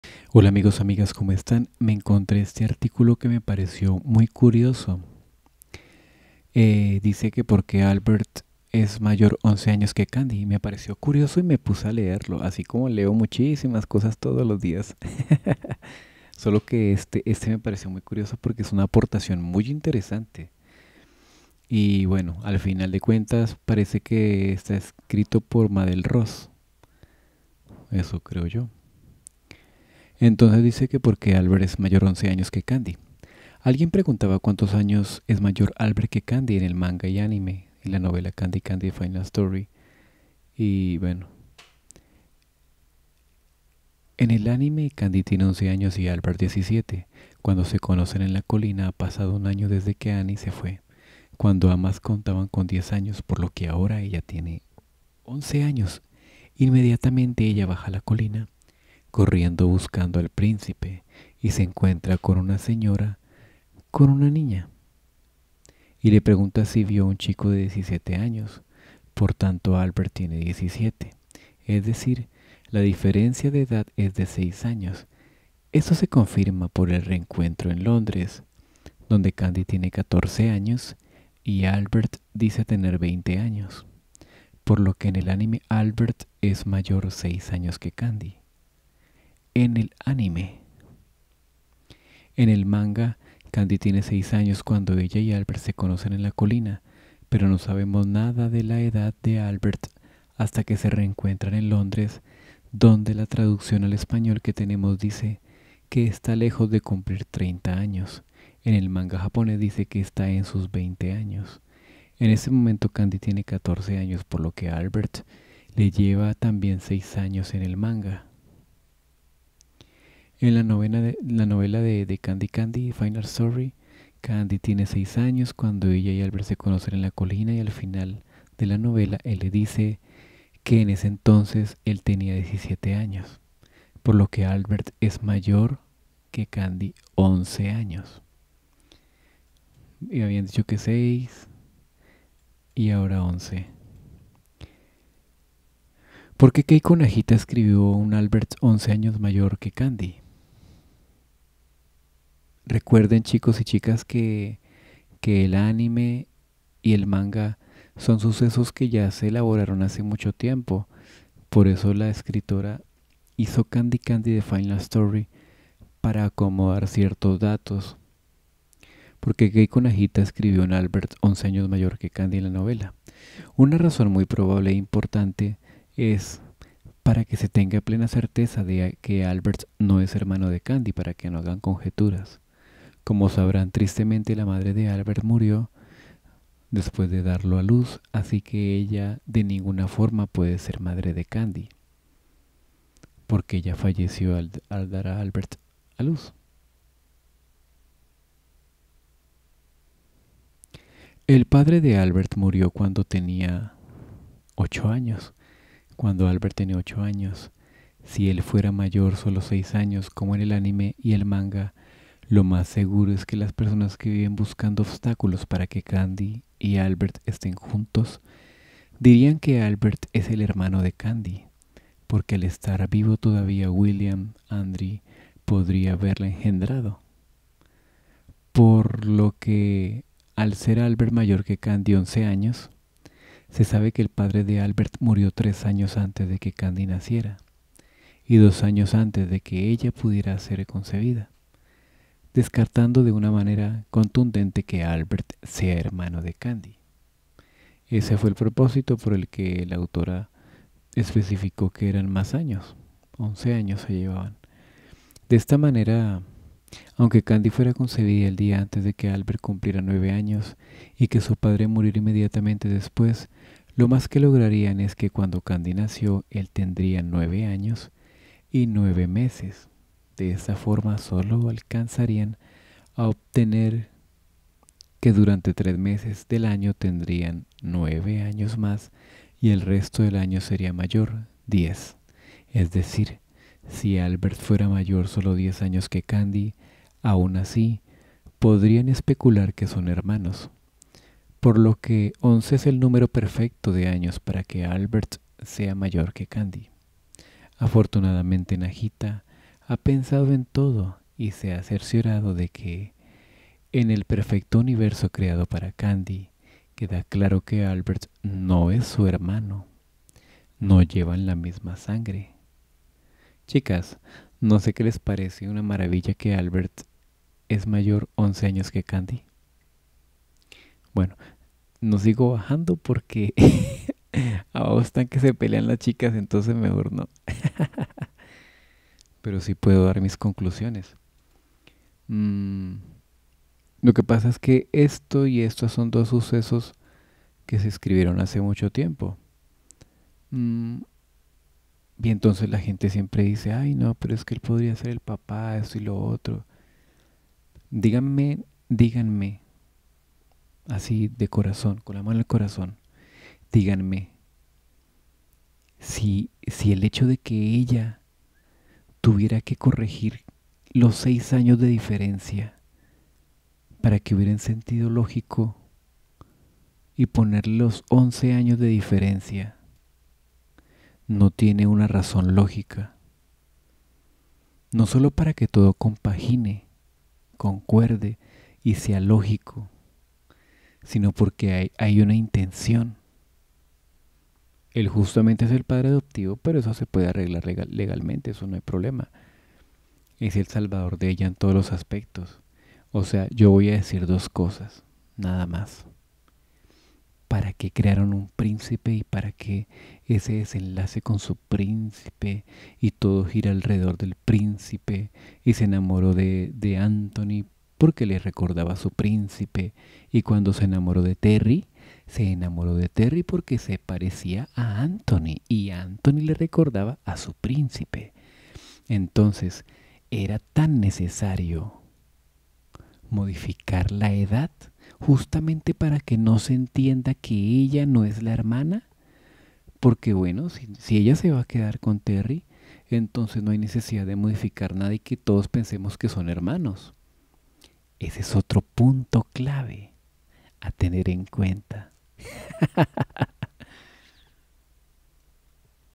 por que Albert le LLEVA 11 años de edad a CANDY
Principe de la colina STEIMAR

Ayuda al Principe. Porfavor. : 3

TERRY - CANDY CANDY FANFIC - FANFICS CANDY CANDY - ANIME